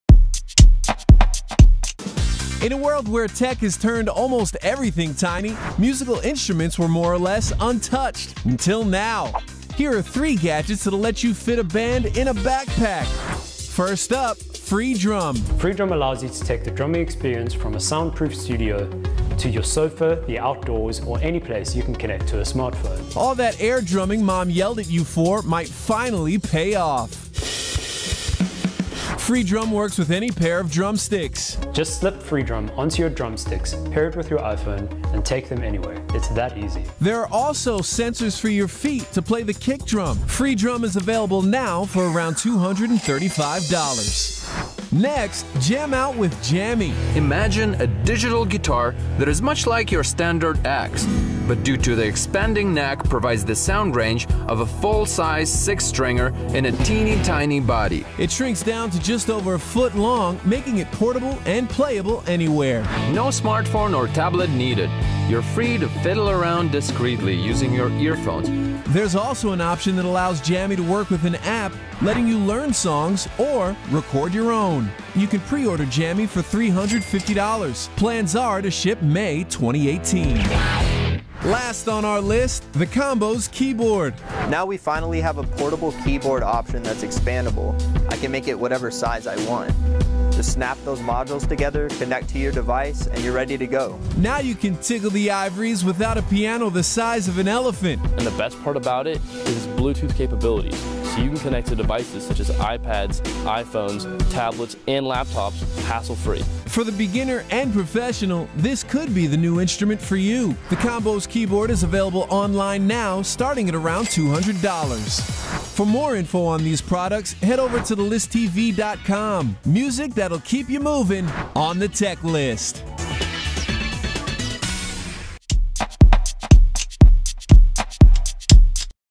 3 On-the-Go Ways You Can Learn a New Instrument
Do you wish you could learn a new musical instrument, but don't think you'll ever find the time? Well, thanks to a magic of technology, you can pick up a new instrument - while you're on the go! Jared Cotter's showing you how to go from a musical novice to an expert with 3 gadgets that'll teach you anywhere: on your commute, on vacation - even at the office (when no one's looking).